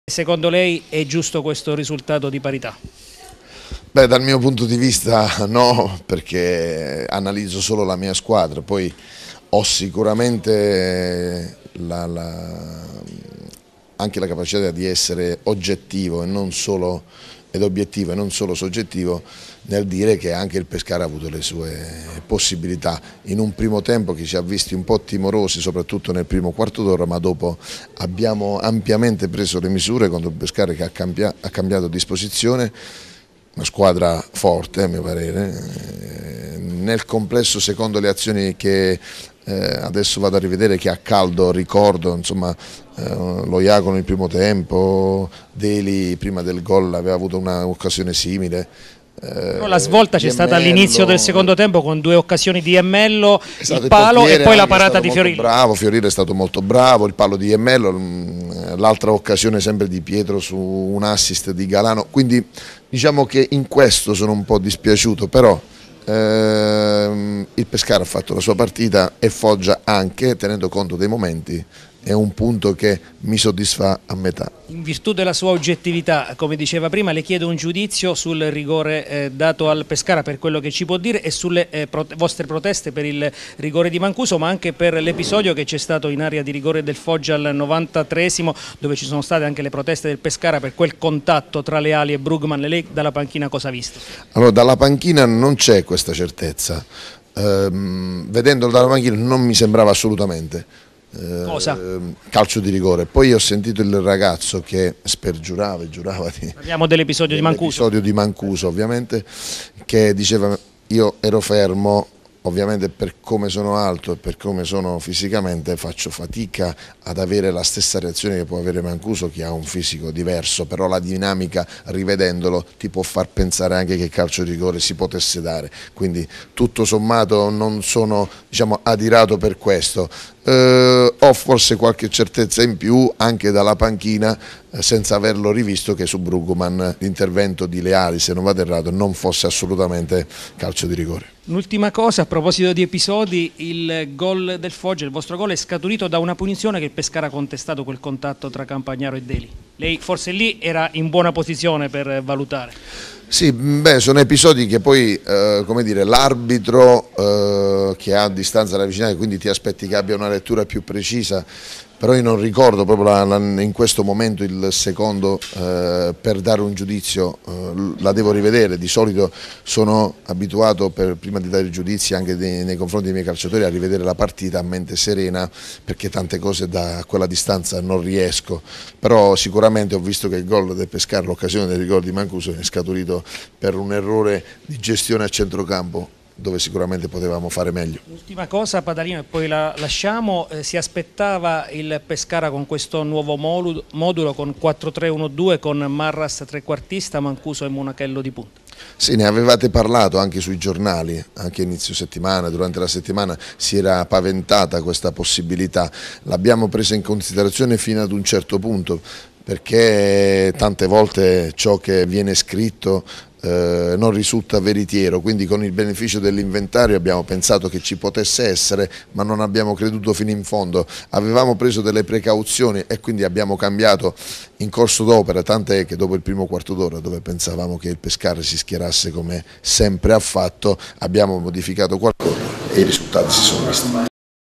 Foggia - Pescara 1-1:Pasquale Padalino
Intervista post gara all'allenatore del Foggia